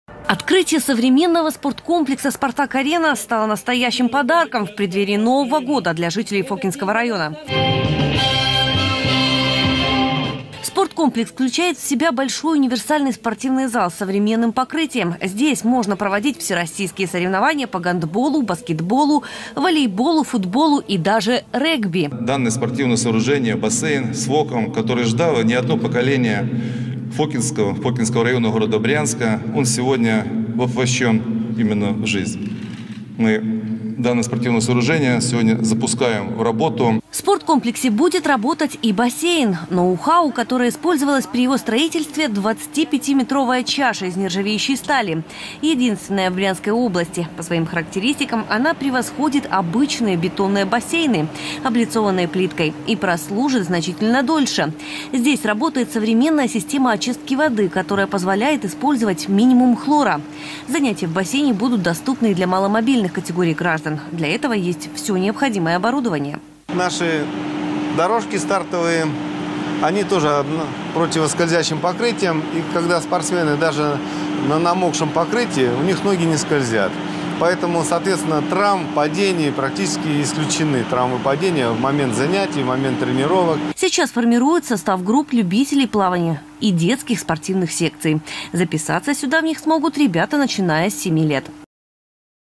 Федеральный телеканал рассказал об открытии в Брянске спорткомплекса «Спартак-Арена»
Об открытии в Брянске современного многофункционального спорткомплекса «Спартак-Арена», которое состоялось накануне, 28 декабря, сообщил федеральный телеканал «Россия 1» (смотреть с 23:26). Напомним, новая арена включает универсальный спортзал с современным покрытием. Здесь можно проводить всероссийские соревнования по гандболу, волейболу, баскетболу, футболу и даже регби, - сообщает автор сюжета для программы федеральной телекомпании.